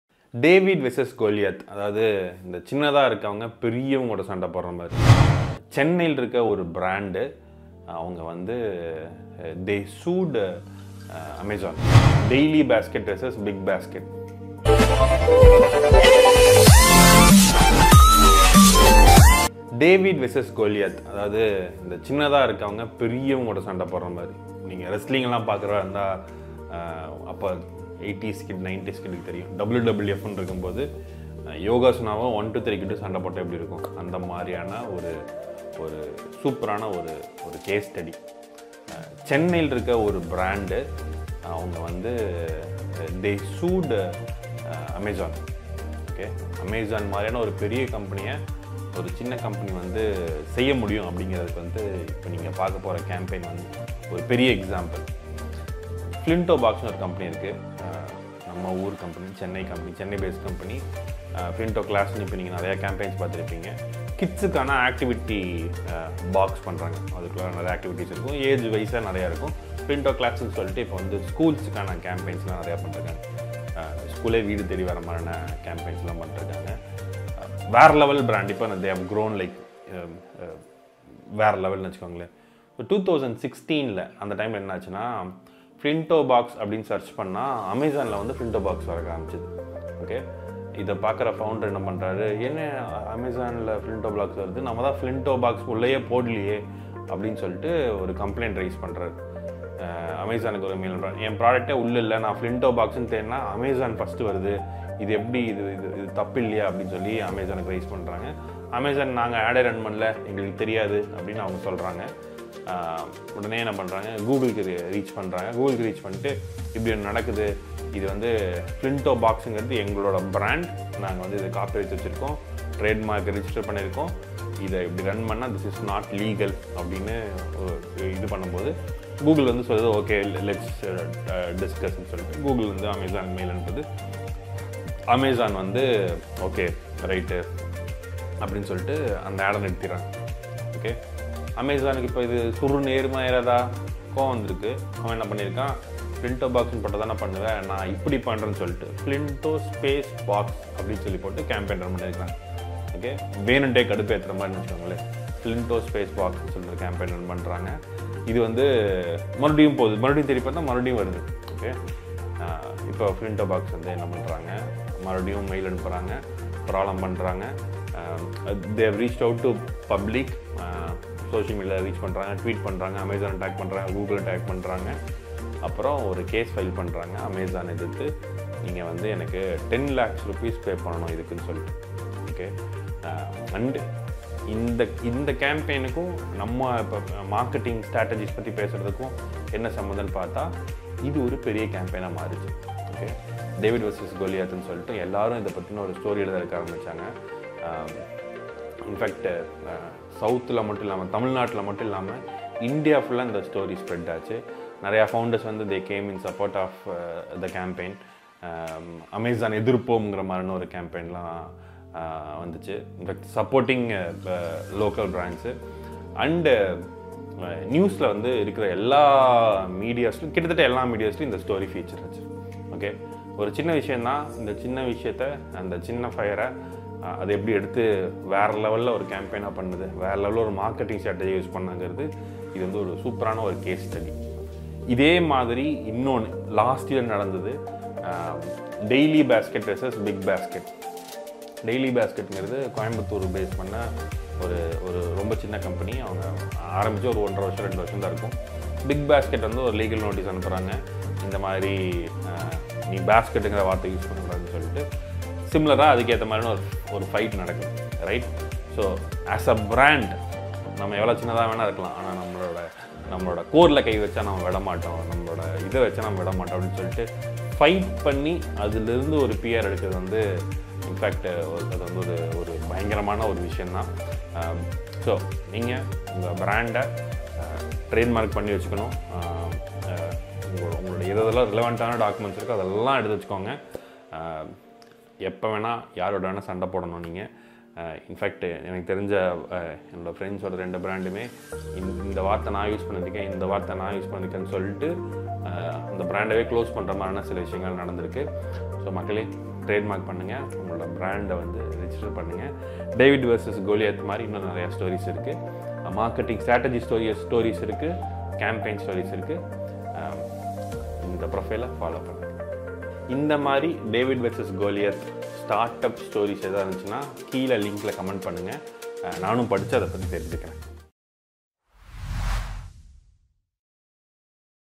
David Vs Goliath | How Chennai based Startup Flintobox Fought with Amazon | Sakthivel Pannerselvam
Vanakkam Makkaley,